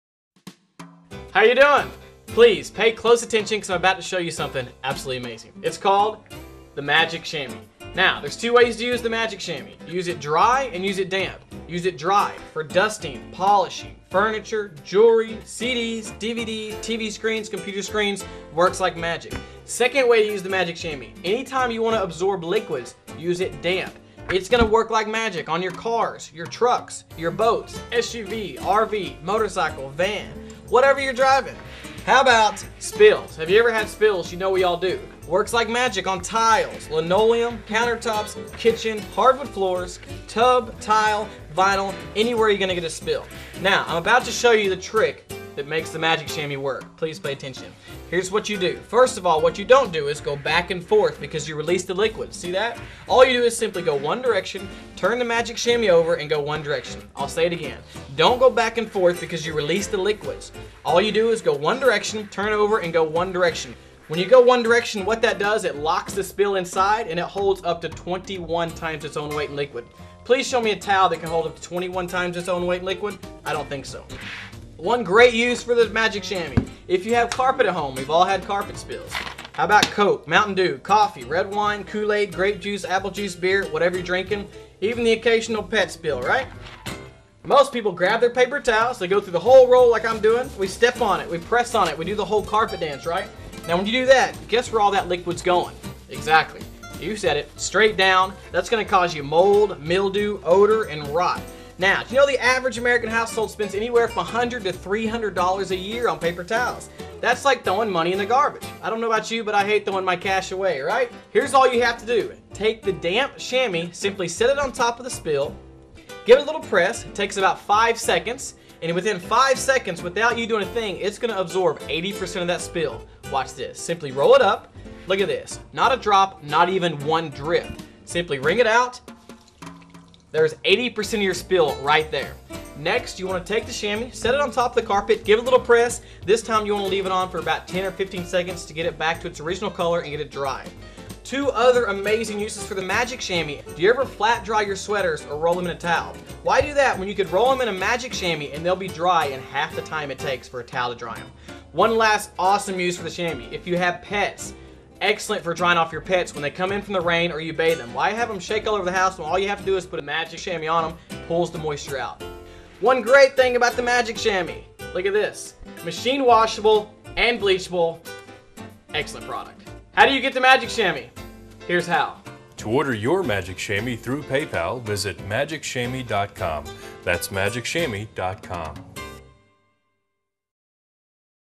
Learn how the Chamois Magic works!
The ChamoisMagic is the Thickest Synthetic Chamois on the Market. The Chamois is great for Vehicles, Carpet, and Spills.